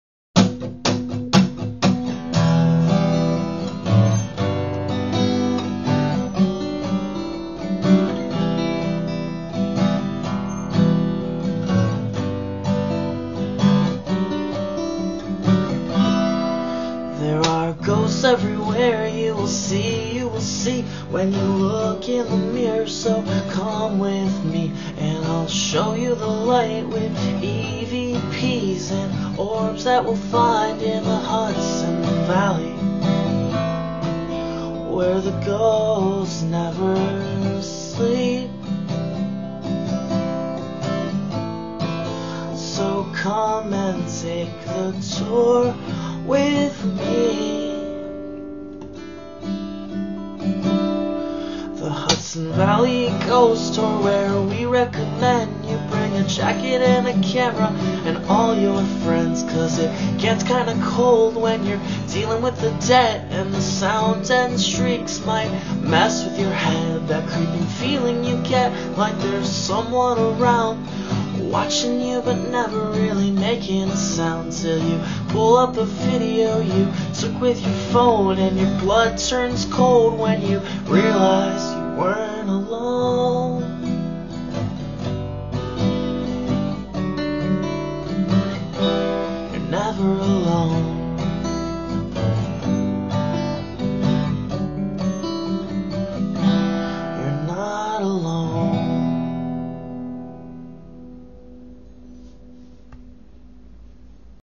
The Hudson Valley Ghost Tour
There are ghosts everywhere you will see,
you will see, when you look in the mirror,
so come with me.
And I'll show you the light with EVPs and
orbs that we'll find in Hudson Valley.
Where the ghosts never sleep
So come take the tour with me.

The Hudson Valley Ghost Tour where we recommend
you bring a jacket and a camera and all your friends,
because it gets kinda cold when you're dealing with the dead,
and the sounds and shrieks might mess with your head.
That creeping feeling that you get like there's someone around,
watching you but never really making a sound,
'til you pull up the video you took with your phone,
and your blood turns cold when you realize you're weren't alone.

You're never alone.